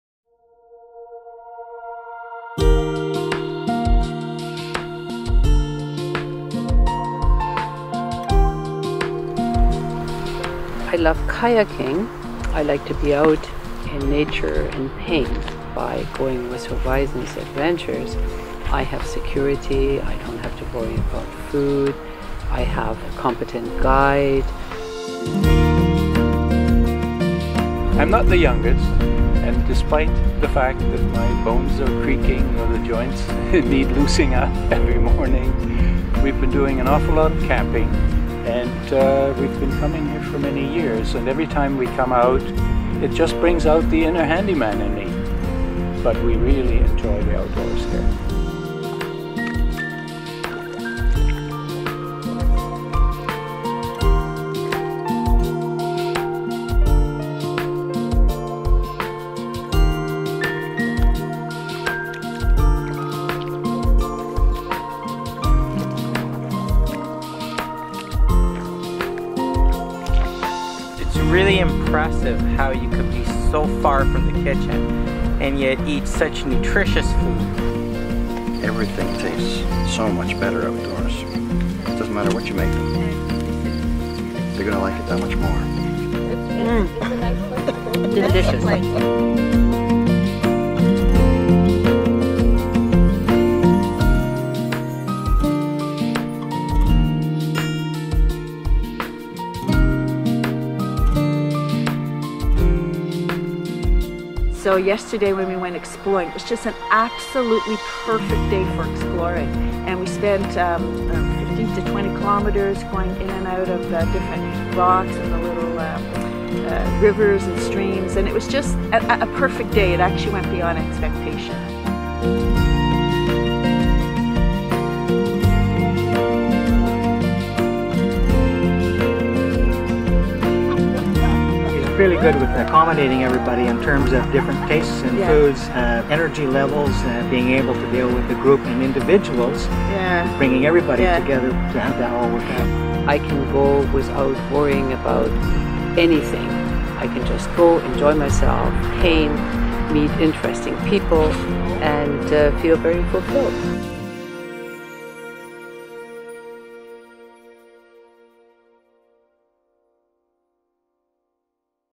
HORIZONS ADVENTURES
This is a short piece I did for a good friend of mine, Rick Wise with Horizons Adventures.

Rick is a Professional Kayaking Guide & Instructor and has been offering amazing trips on Ontario's Great Lakes for over a decade.